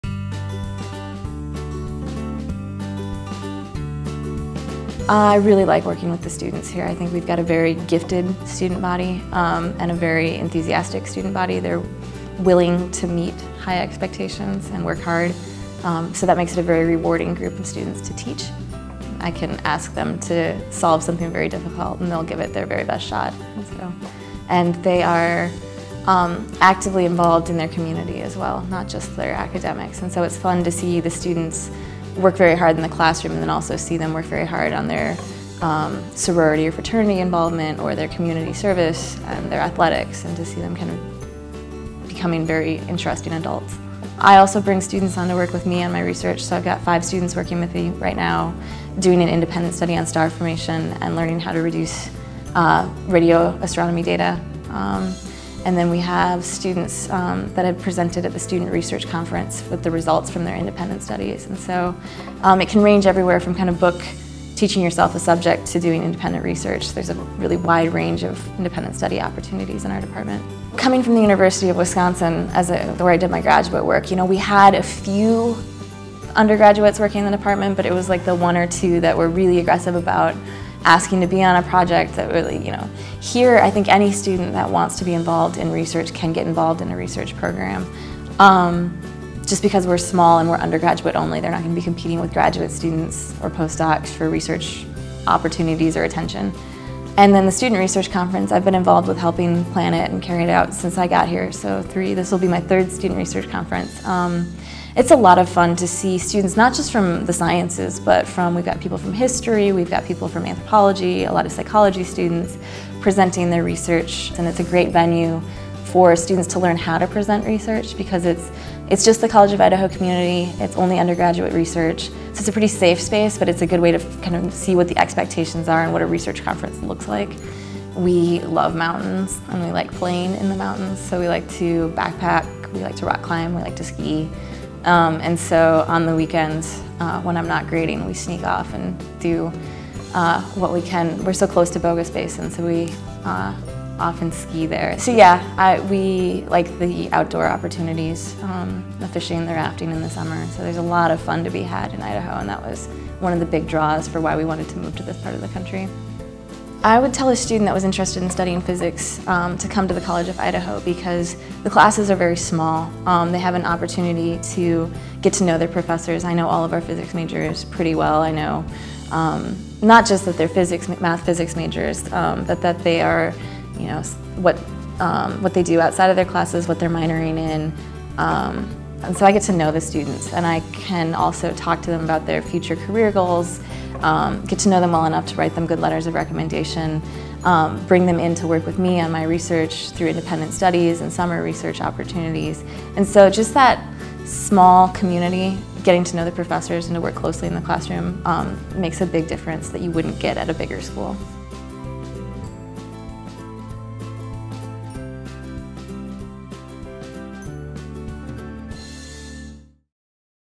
The College of Idaho: Katie Devine
Katie Devine, professor of mathematics and physical sciences, discusses why she enjoys teaching at The College of Idaho and opportunities for students to work with faculty on undergraduate research projects. Professor Devine also talks about her love for the numerous opportunities for outdoor recreation in Idaho. For more information about studying at The College of Idaho,